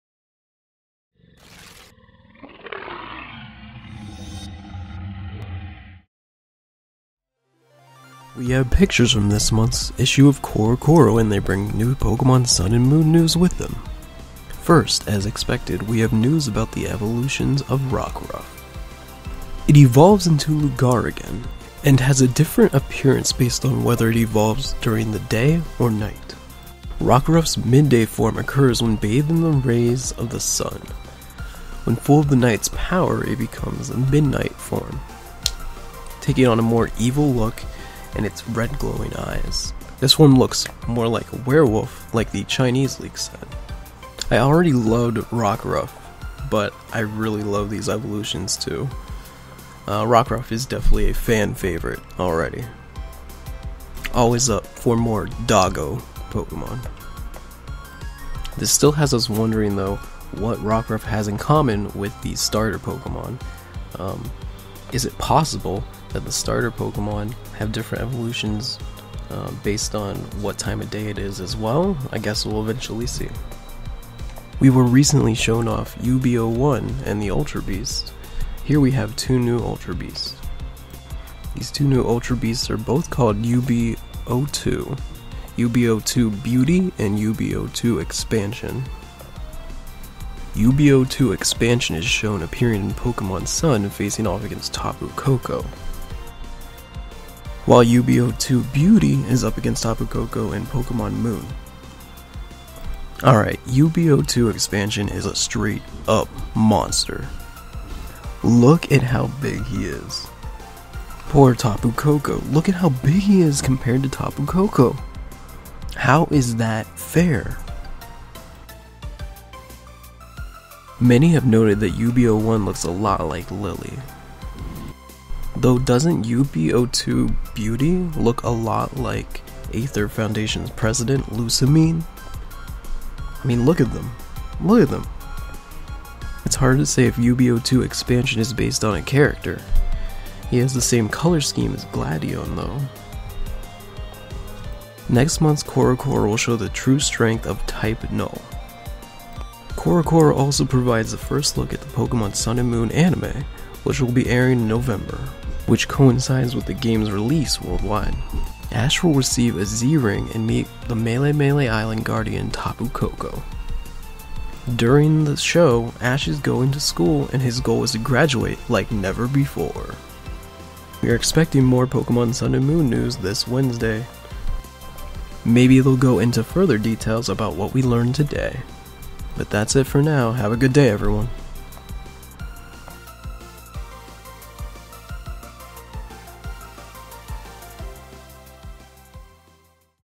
New Pokemon Revealed! Rockruff Evolution and Ultra Beasts! Pokemon Sun and Moon
September's CoroCoro revealed information about new pokemon and new Ultra Beasts!

Sorry about my voice, dealing with a sore throat.